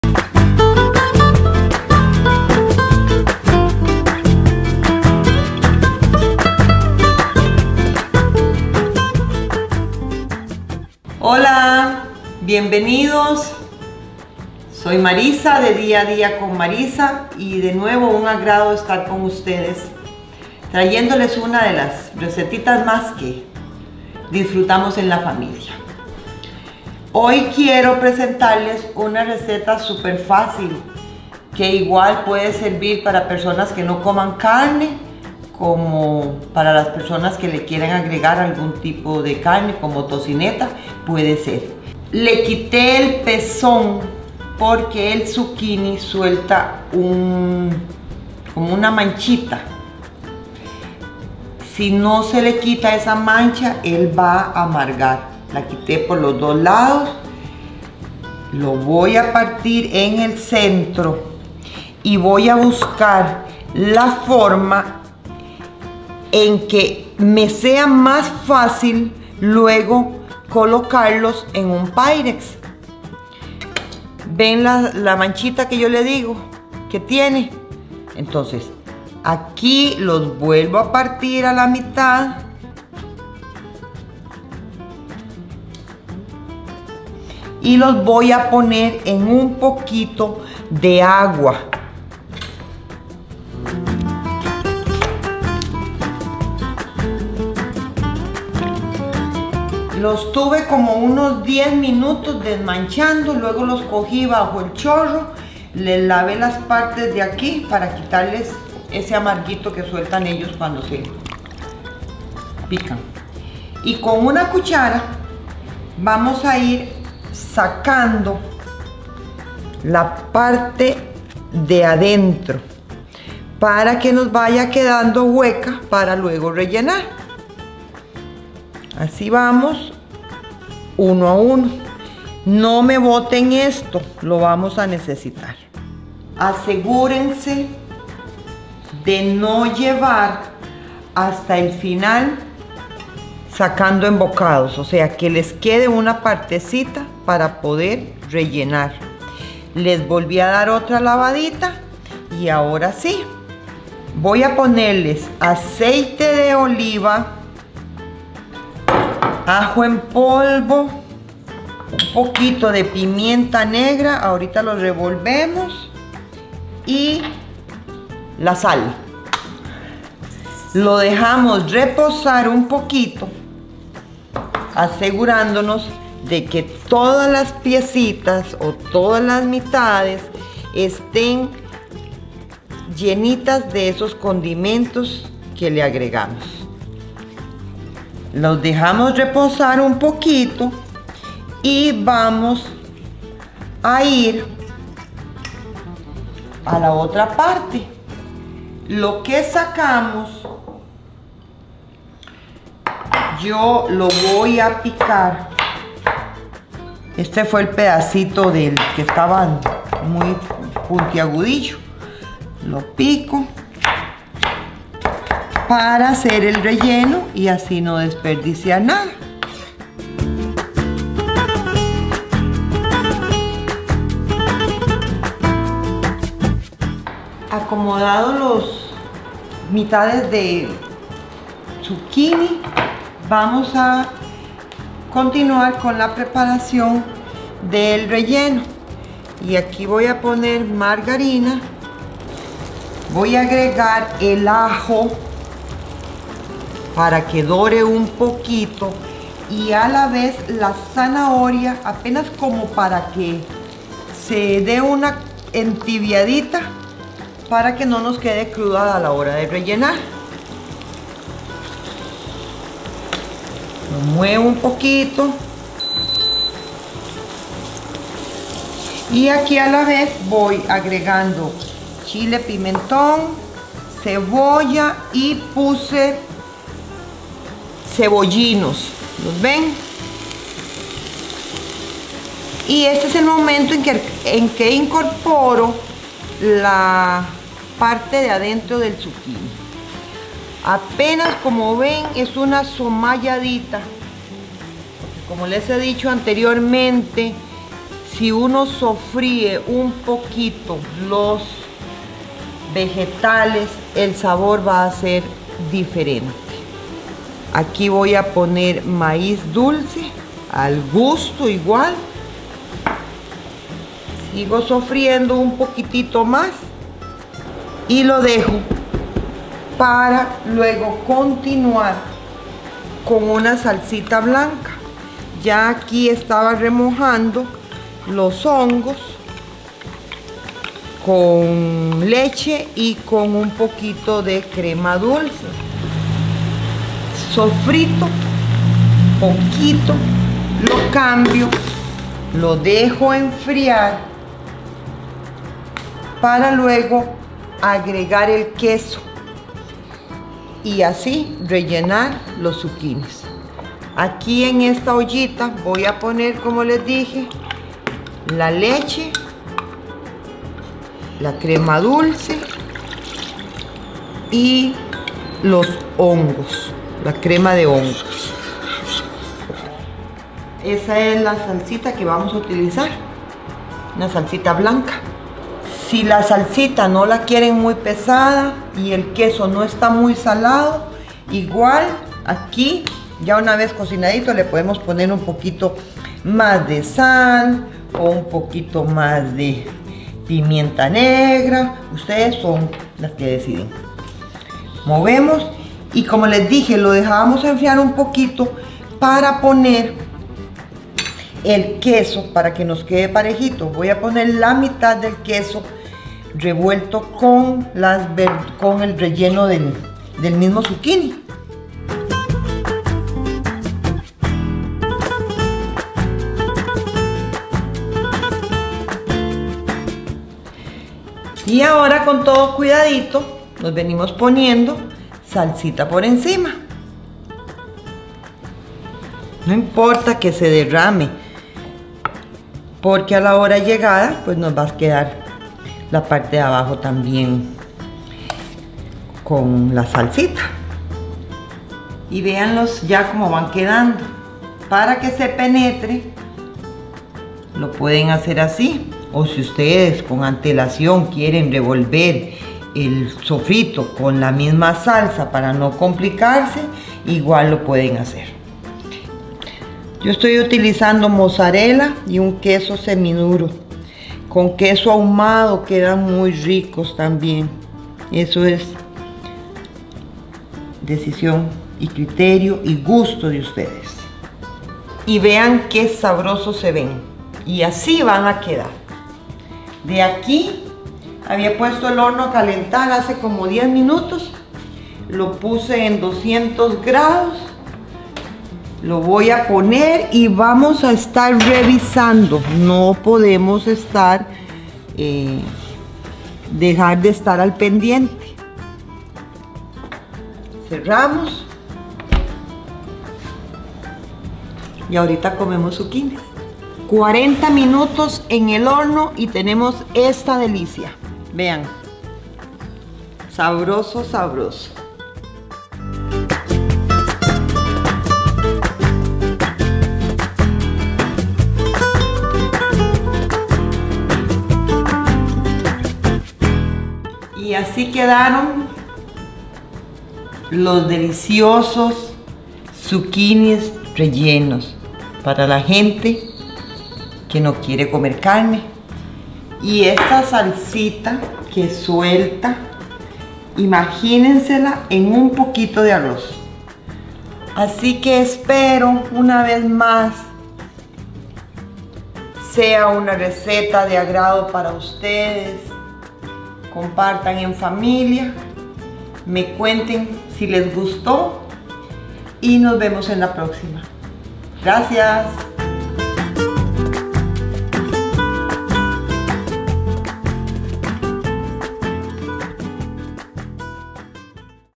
Prepare unos deliciosos zapallos o zucchinis rellenos
Ya saben que me gusta compartirles todos mis secretitos de cocina y en esta receta les enseño como hacer para desmanchar nuestros zapallitos antes de cocinarlos para que nos queden unos zapallos rellenos absolutamente deliciosos y sin ningún amargo

Vamos a necesitar:
🥒 3 Zucchinis o Zapallos (calabacín)
🧈 1 ajo fresco
🥕 ½ zanahoria
🥕 1 cebollino
🥕 ½ cebolla
🌽 ½ chile morrón (o chile dulce)
🌽  ½ taza de maíz dulce o más gusto
🌶2 cucharaditas de sal

Para la salsa blanca:
🍄 4 cucharadas (1 paquete) de Crema de Hongos
🍄 1 taza de leche
🥛 1 Taza de crema espesa (crema dulce)
🧀 2 tazas de queso para derretir

Procedimiento:

1. Para desmanchar los zucchinis vamos a remover el pezón, partir a la mitad a lo largo y luego partimos cada mitad a lo largo y los vamos a dejar en agua fría por 10 minutos. Pasado este tiempo los lavamos bien.

2. Con una cuchara vamos a ir sacando bocados del centro de cada pieza del zapallo para hacer espacio para el relleno. Guardamos la carne del zucchini para utilizar como parte del relleno.

3. Lavamos las piezas nuevamente y en un tazón las condimentamos con el aceite de oliva, ajo en polvo, pimienta negra y sal y nos aseguramos que todos nuestros zapallos queden bien condimentados. Acomodamos en un molde extendido y dejamos de lado.

4. Preparamos el relleno. En una sartén agregamos la mantequilla junto con la zanahoria y el ajo fresco y dejamos sofreír  por 2-3 minutos, agregamos luego la cebolla, cebollín, chile dulce y el centro de los zucchinis y continuamos sofriendo todo. Por último agregamos el maíz dulce. Dejamos de lado.

5. En una cacerola mediana revolvemos los ingredientes de la crema blanca de hongos y ajustamos la cantidad de sal y dejamos cocinar hasta que la salsa se espese.

6. Agregamos la mitad del queso a nuestro relleno y revolvemos. Rellenamos todos nuestros zucchinis y agregamos más queso y la salsa blanca por encima de cada uno. Llevamos al horno a 175 ℃ ó 350 ℉ por 35-40 min o hasta que el queso se haya gratinado.

Para los que me han preguntado, las hermosas ollas y platos glaseados que uso los pueden conseguir en Cerámica Artística Salitral, les dejo el enlace acá

Muchas gracias por ser parte de la comunidad, síganme en Facebook, donde día a día comparto otros secretitos de la cocina 😉 Les dejo mi enlace a continuación: